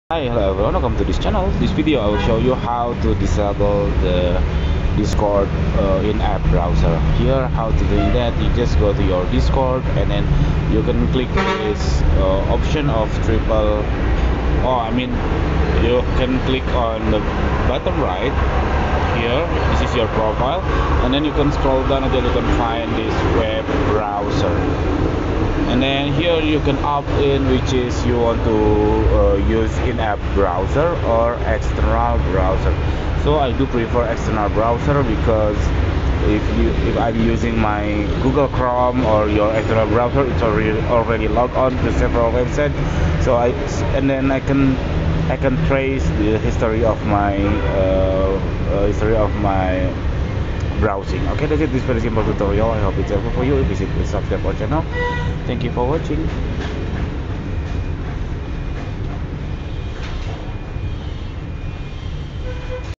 How to Disable In-App Browser of Discord | Open Link in Discord
Here if you wanro open link on Discord using your own Google Chrome or Safari, instead of Discord in-app browser